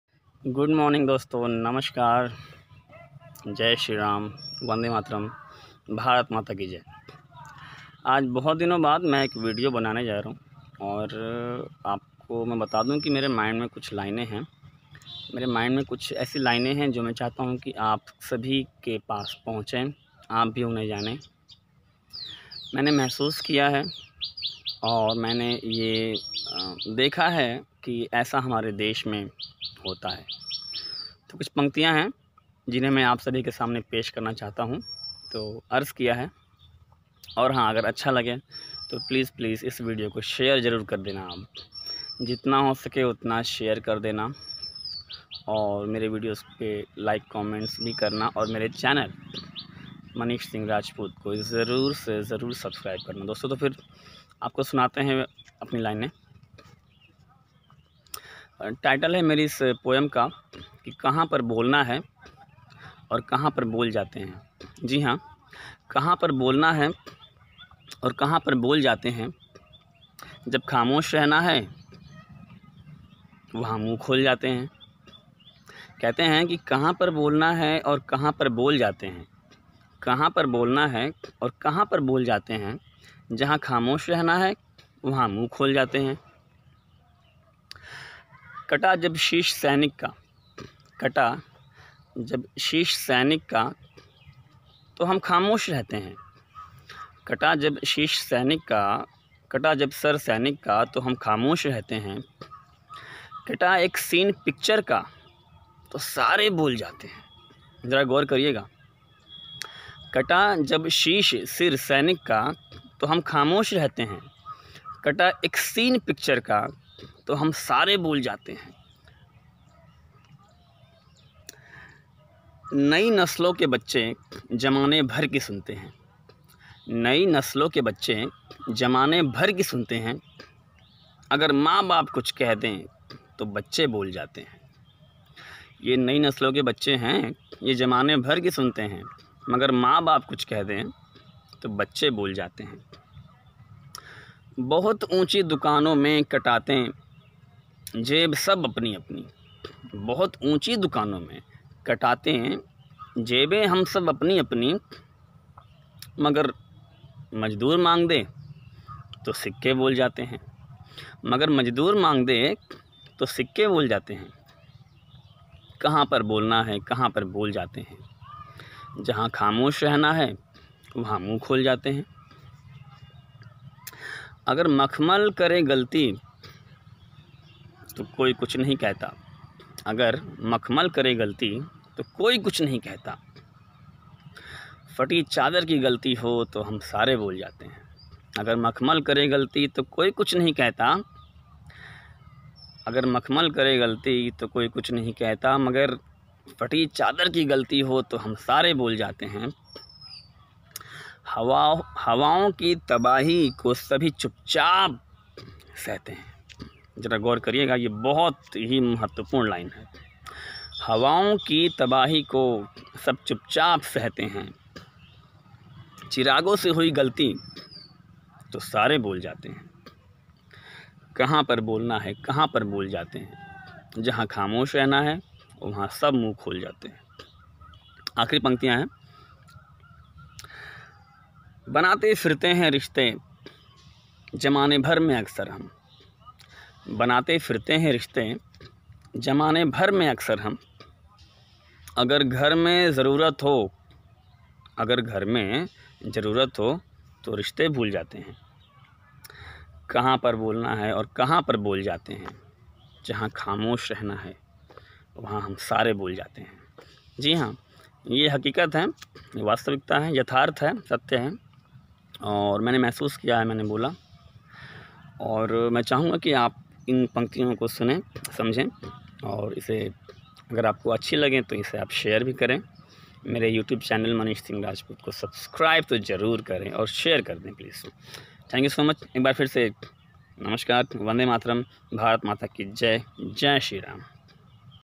Kaha_Par_Bolna_Hai_Kaha_Par_Bol_Jate_Hai
A Great poem by Anupam Kher sir.I tried in my voice ,in my style..